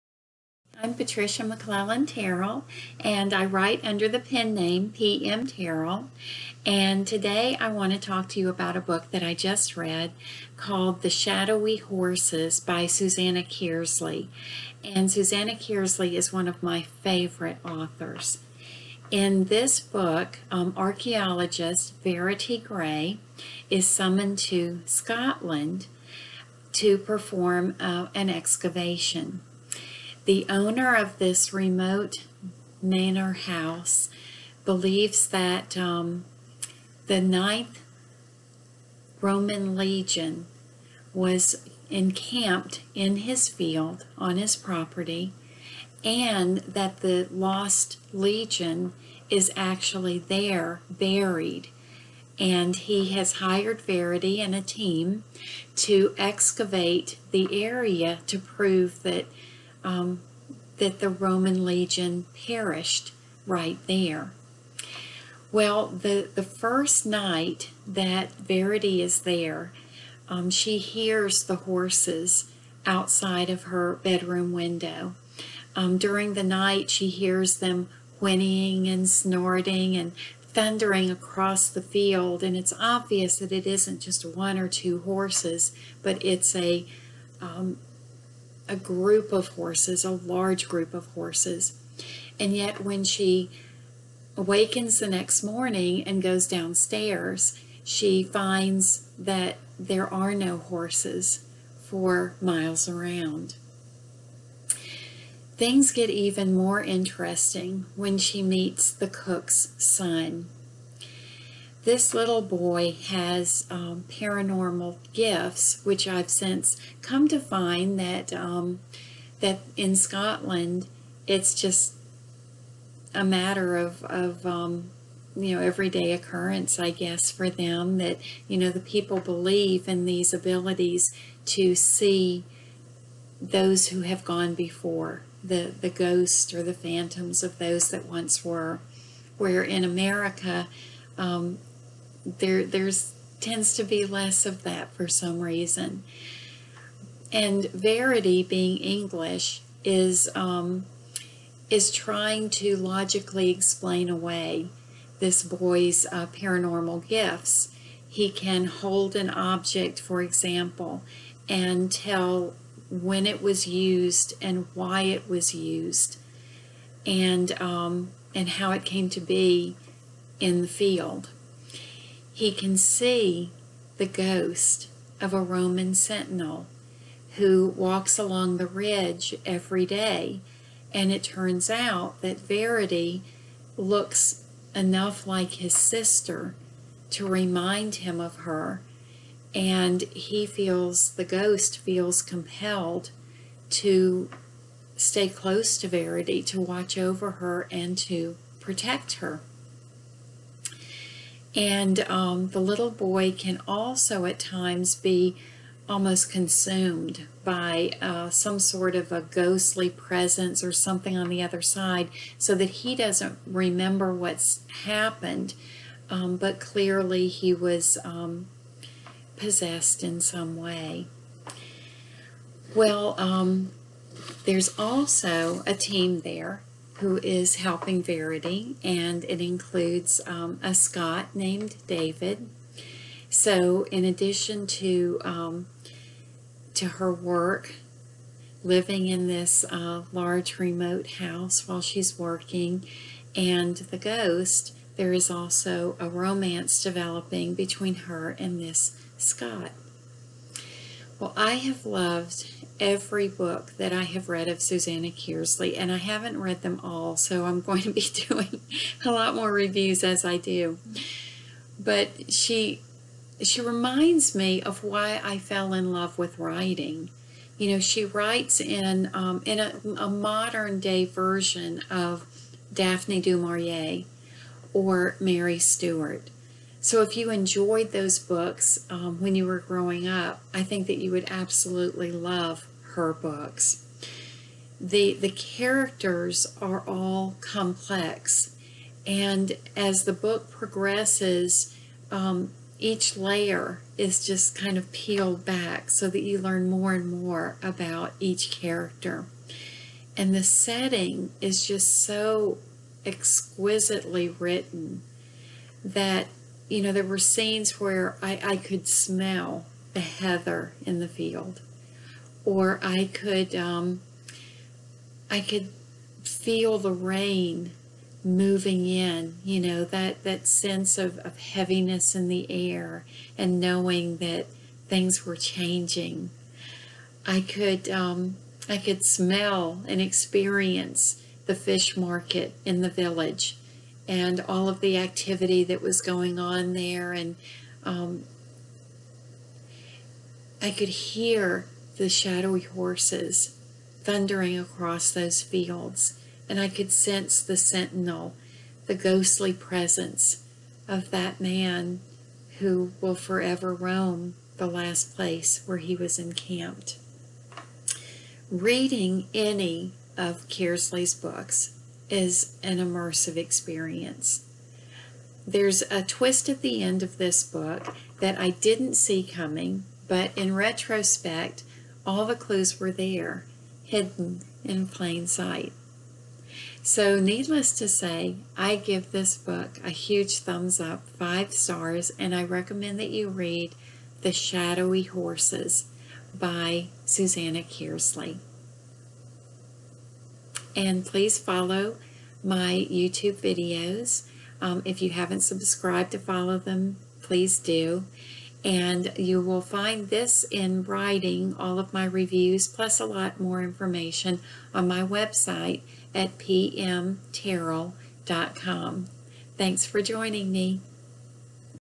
The Shadowy Horses
A review of The Shadowy Horses by Susanna Kearsley.